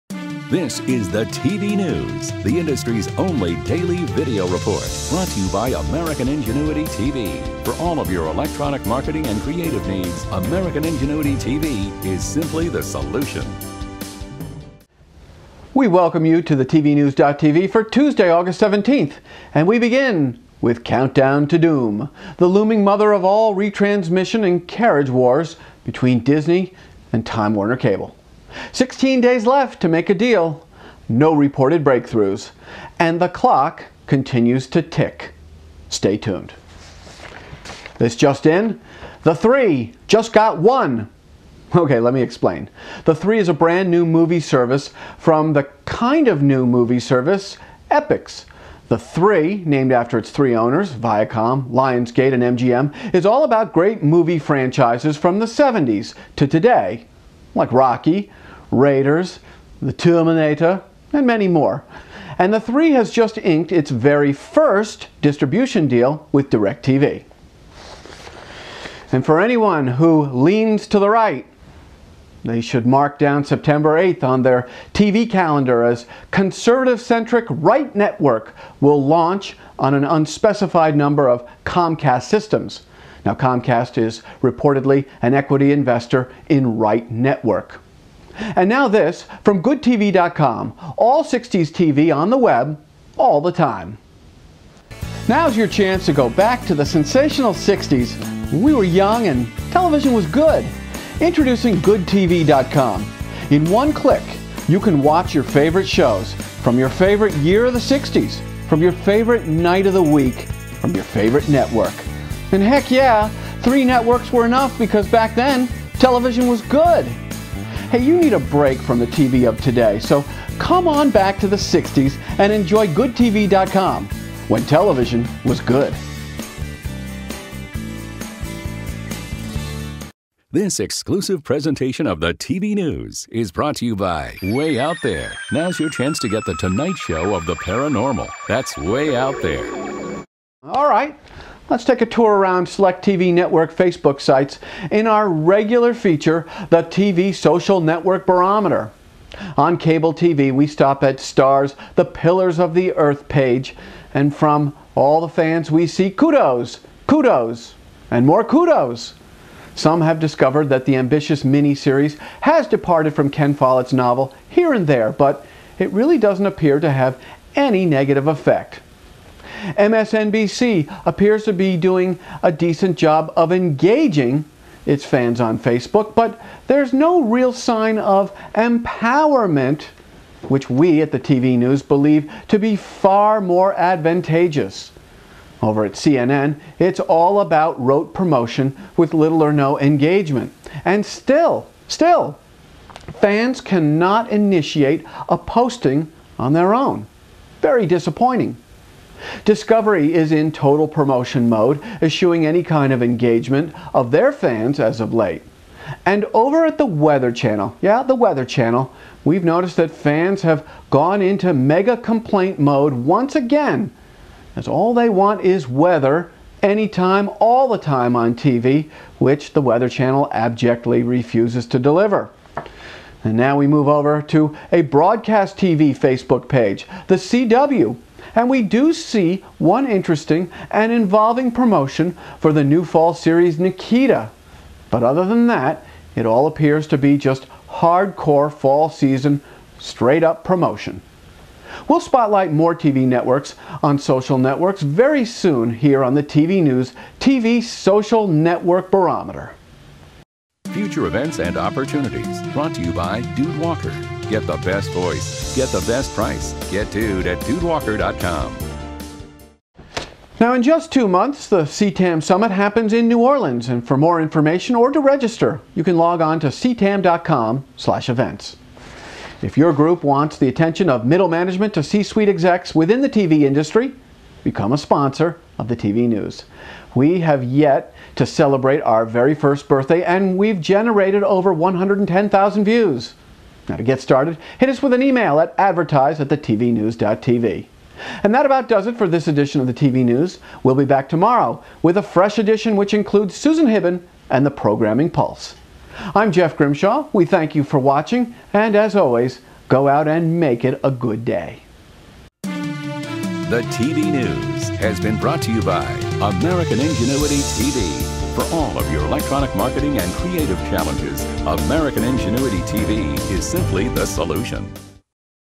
See Our TV / Social Network Barometer For CNN, Discovery & More. TV Industry News 8.17.10
Our "TV / Social Network Barometer" will give you a read on how CNN, Discovery, MSNBC, The CW and more networks are doing with social media.  Is the market ready for yet another premium movie channel - Viacom, Lionsgate, MGM and DirecTV thinks so!  Get ready to feel "All Right" on September 8 because that's when conservative-centric Right Network launches.  Plus, the countdown continues to the Disney / Time Warner Cable retransmission & carriage war.